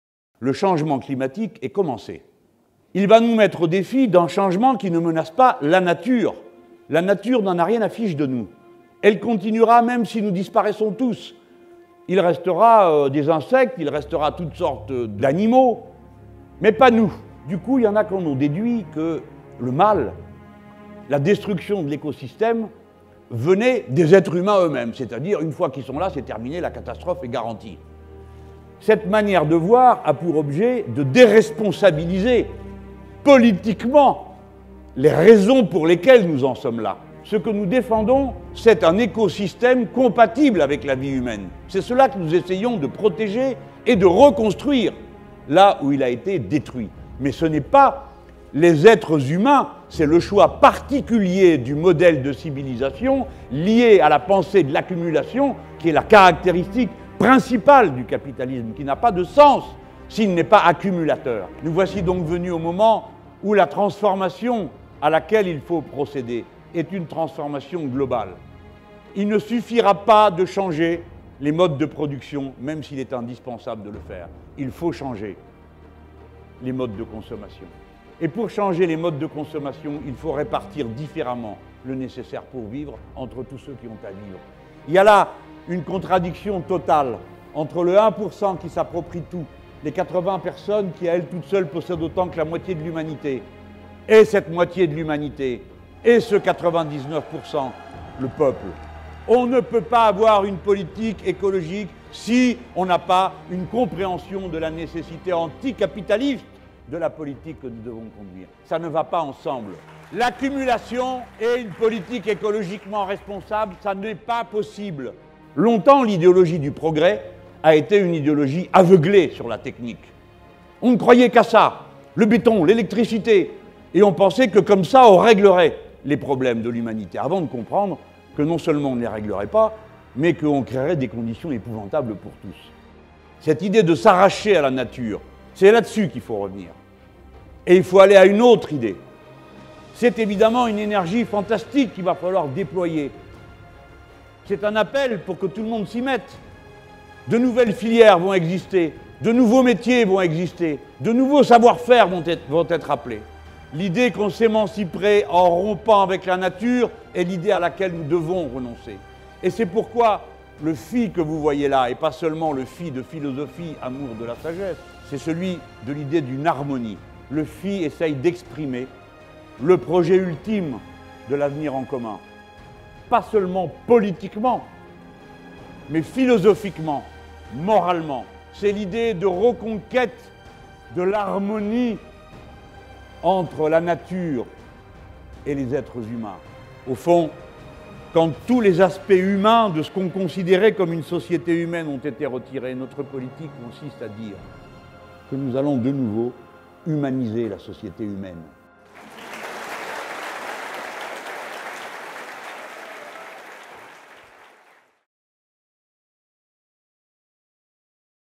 Le changement climatique est commencé
Intervention de Jean-Luc Mélenchon du samedi 25 février 2017 en conclusion de la Journée de l'Écologie de la France insoumise.

Le changement climatique est commencé. C’est le système capitaliste qui en est la cause. Pour faire la bifurcation écologique, il faut tout changer dans nos manières de produire et de consommer. Et partager la richesse.